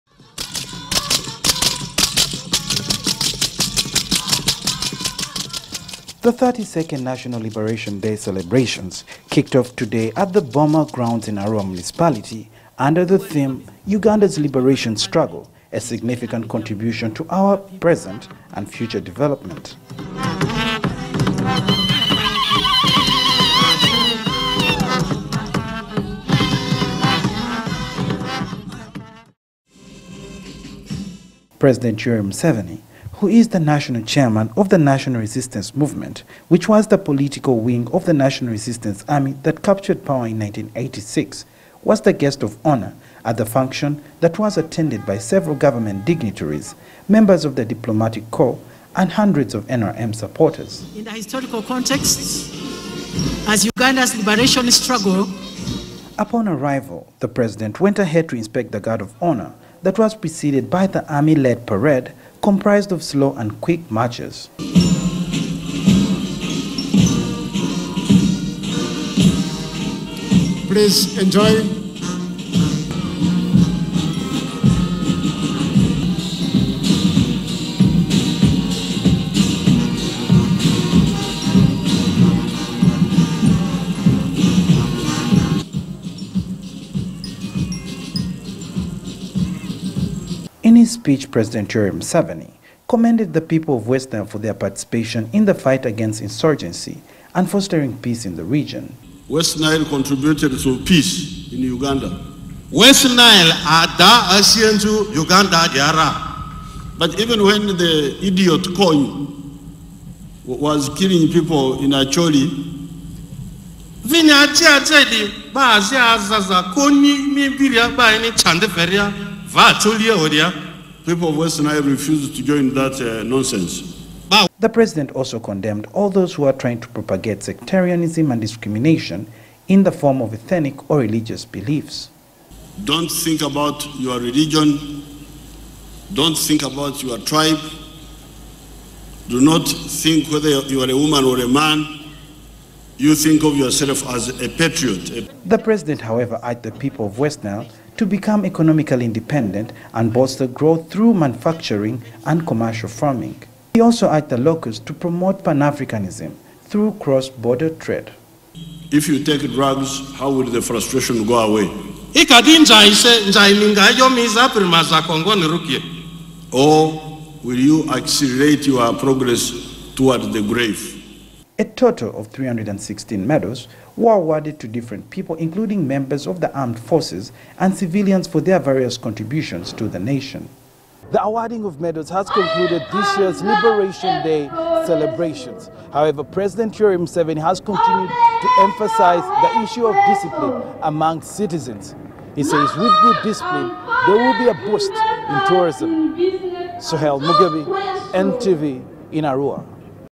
Museveni commends the people of West Nile for fighting insurgencies
President Yoweri Museveni has commended the people of West Nile for their role in the fight against the various insurgencies that plagued the region and their role in bolstering peace.

The President made these remarks while officiating over the National Liberation Day Celebrations in Arua District today.